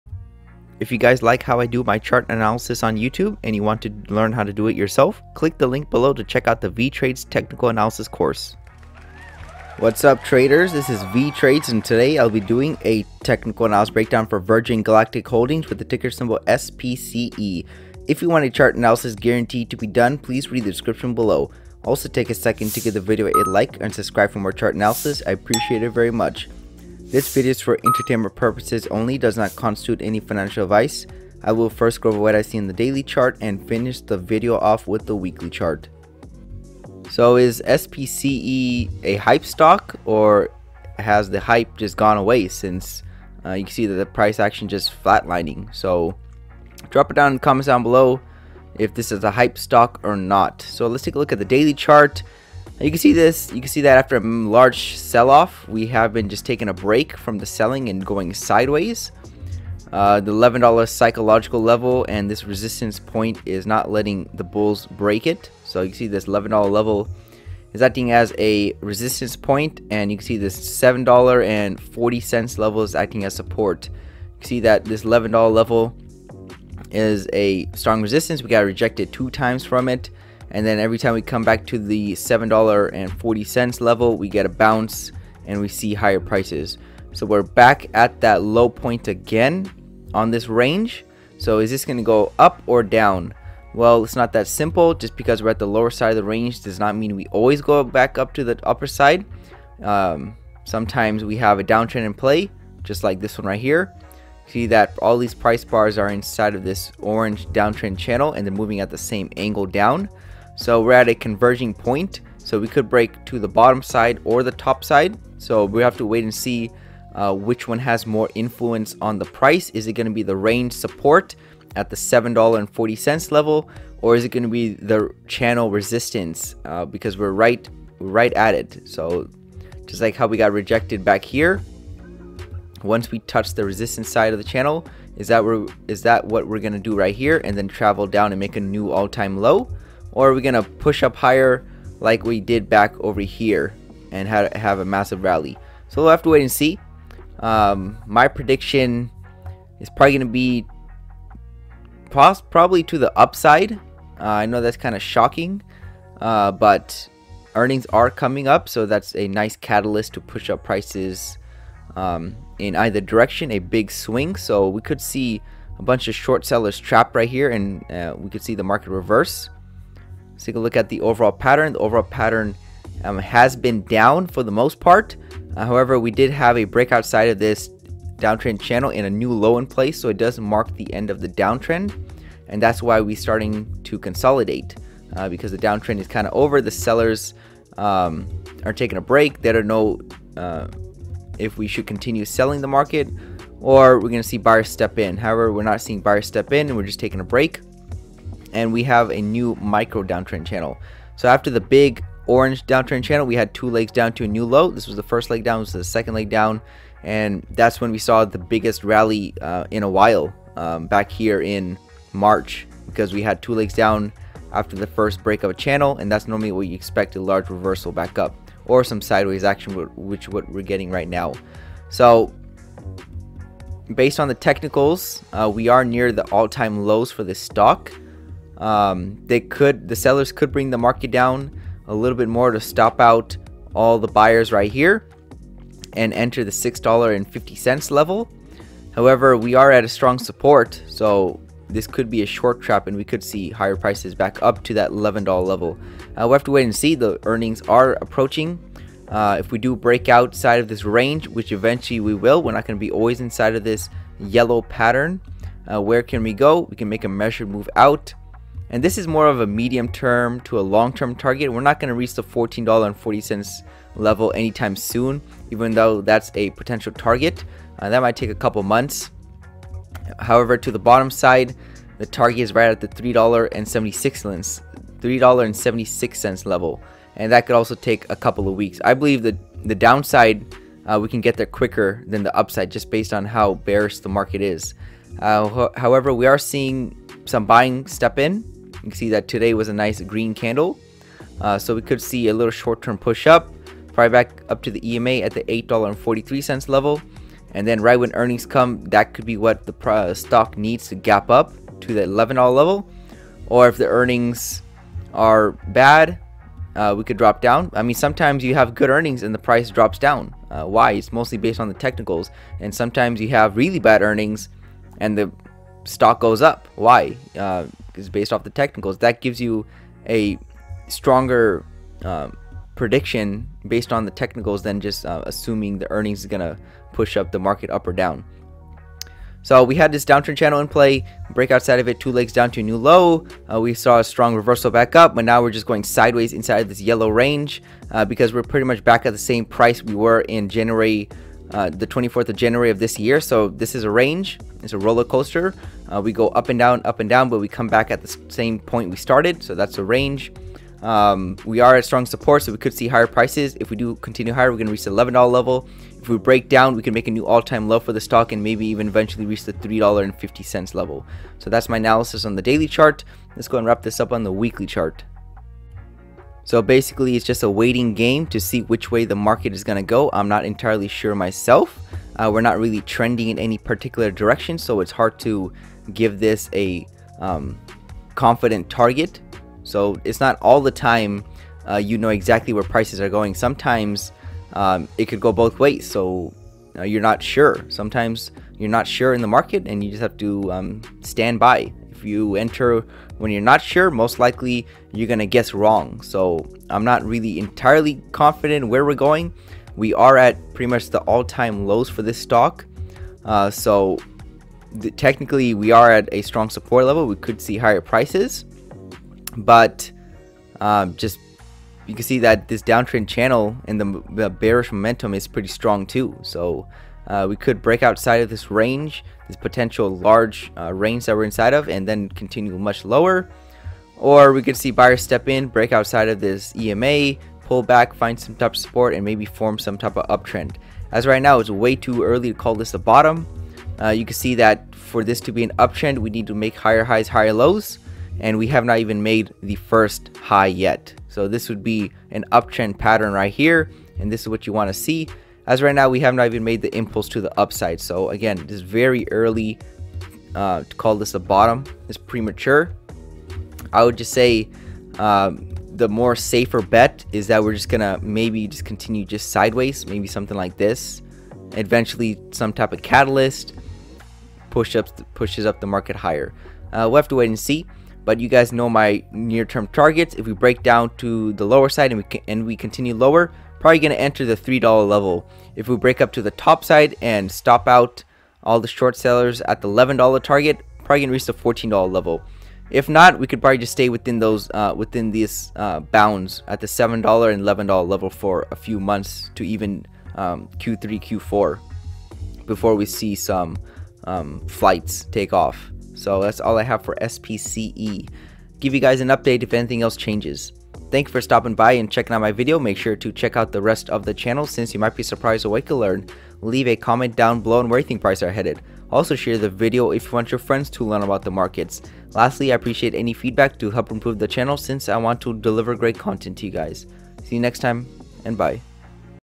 Virgin Galactic Holdings Stock (SPCE) | Price Predictions Using Technical Analysis.
⌨ 3. Also, please post a comment under my recent video stating you left a donation, so I do not miss it.
Hello YouTube❗,

This is my 355th video on the channel. I will be doing a Technical Analysis breakdown for Virgin Galactic Holdings.